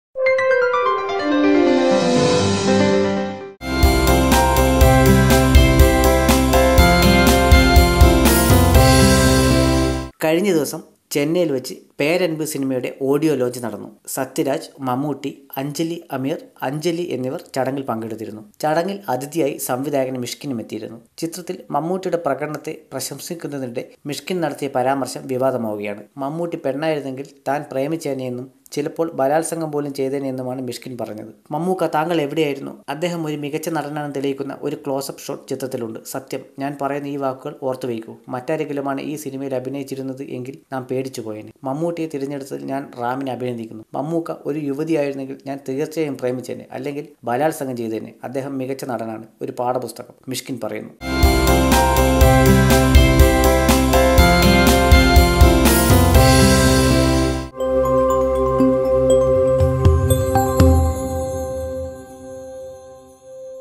Director Mishkin Controversy Statement About Mammootty | YOYO TV Malayalam
At a time when there’s increasing focus on the film industry’s treatment of female actors, a distasteful comment from director Mysskin at the audio launch of Peranbu on Sunday, has attracted the ire of social media.
The ace filmmaker was addressing the audience at the audio launch of Ram’s Peranbu which stars Mammootty in the lead. After complimenting Ram’s work, the director was gushing about Mammukka’s acting in the film, when he quipped, “If I were a girl, I would have loved him.” Seemingly not content with the reaction the comment, he said, “If I were a girl, I would have raped him.”
Shockingly, the comment was welcomed by laughter and applause by the people present at the function, including many popular film personalities. The director finished off his speech by saying, “You have to create a controversy somehow to make this film a big hit and with that, my job here is done.”
One doesn’t know whether the rape joke was premeditated, or whether Mysskin was perhaps trying to play down the seriousness of what he had just said.